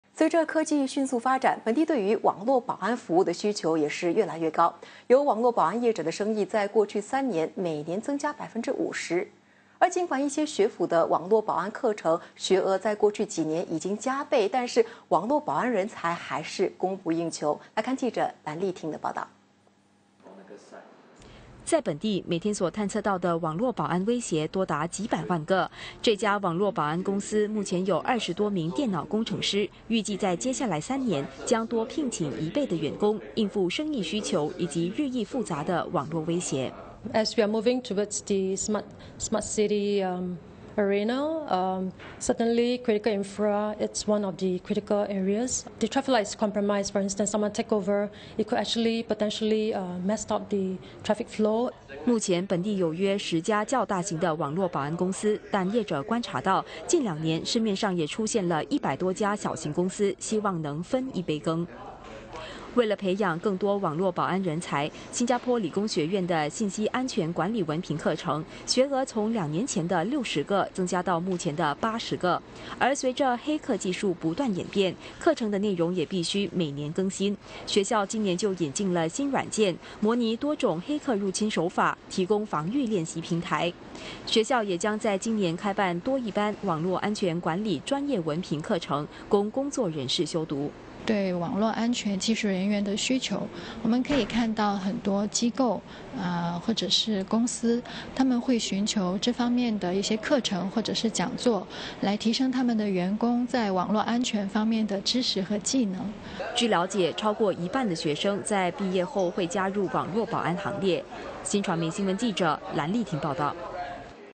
网络保安服务需求增高 专业人才供不应求
随着科技迅速发展，本地对网络保安服务的需求越来越高。有网络保安业者的生意在过去三年，每年增加50%。而尽管一些学府的网络保安课程，学额在过去几年已加倍，网络保安人才还是供不应求。